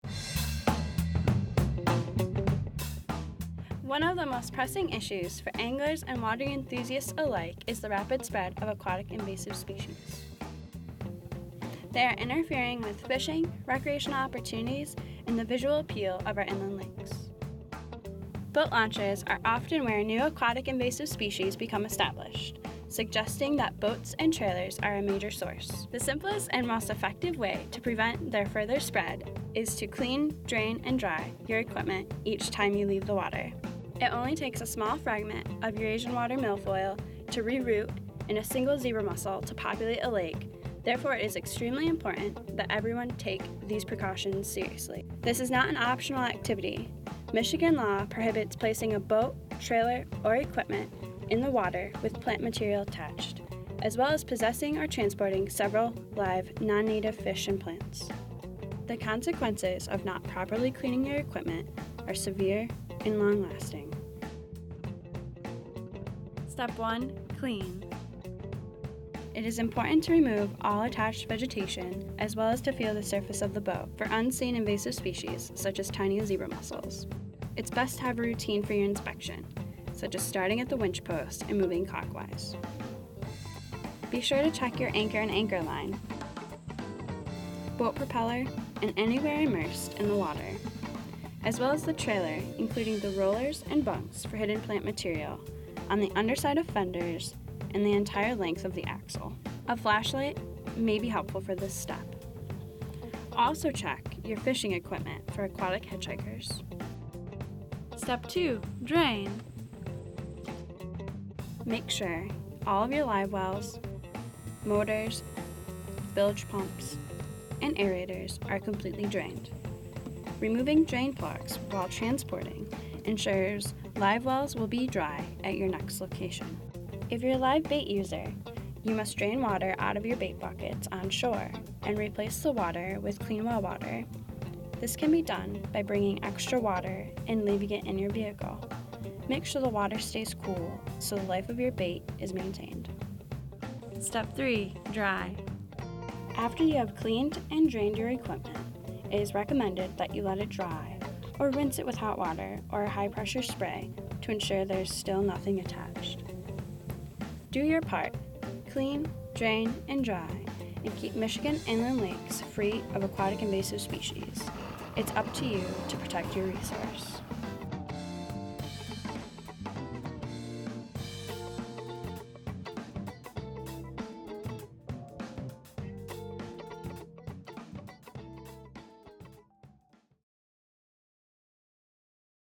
Michigan Clean Boats, Clean Waters: Clean, Drain, Dry!
Boaters are encouraged to Clean, Drain, and Dry their equipment to reduce the spread of aquatic invasive species between water bodies.  This short video shows how quick and simple this can be! The Michigan Clean Boats, Clean Waters program aims to educate boaters and anglers about the risks that invasive species pose to Michigan's lakes and rivers, and partner with boaters to reduce their spread.